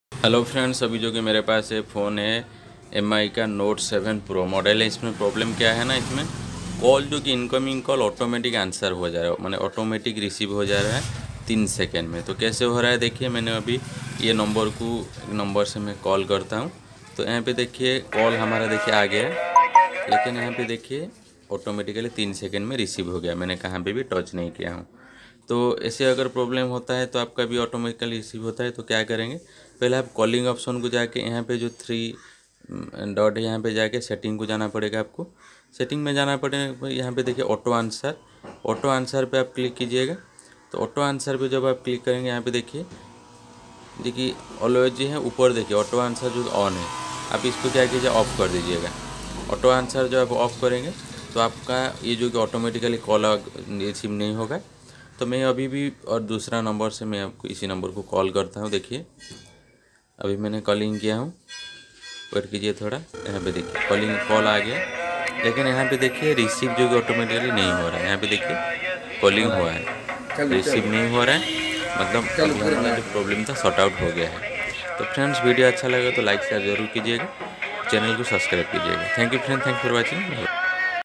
Mi Note 7 Pro Call Auto Answering Solution || Mi Auto Call Answering Settings
In this video we know how to turn off mi note 7 pro auto call Answering setting. This tricks work in all mi models.